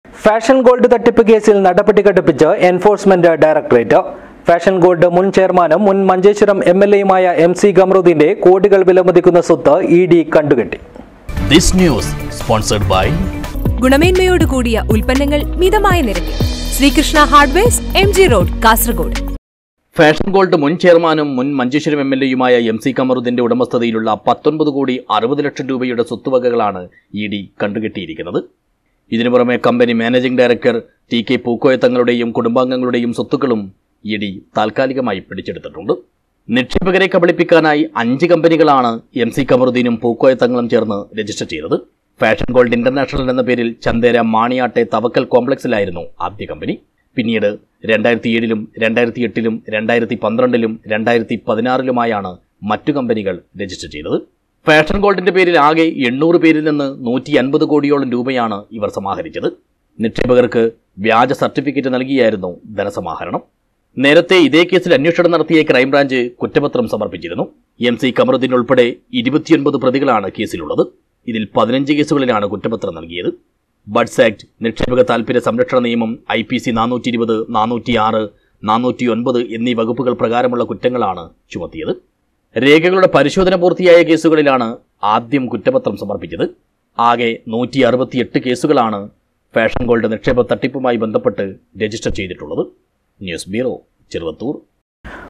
ഫാഷന്‍ ഗോള്‍ഡ് തട്ടിപ്പ് കേസില്‍ നടപടി കടുപ്പിച്ച് എന്‍ഫോഴ്സ്മെന്റ് ഡയറക്ടറേറ്റ്.
ഫാഷന്‍ ഗോള്‍ഡ് തട്ടിപ്പ് കേസില്‍ നടപടി കടുപ്പിച്ച് എന്‍ഫോഴ്സ്മെന്റ് ഡയറക്ടറേറ്റ്. ഫാഷന്‍ ഗോള്‍ഡ് മുന്‍ ചെയര്‍മാനും മുന്‍ മഞ്ചേശ്വരം എംഎല്‍എ-യുമായ എം.സി കമറുദ്ദീന്റെ കോടികള്‍ വിലമതിക്കുന്ന സ്വത്ത് ഇഡി കണ്ടുകെട്ടി.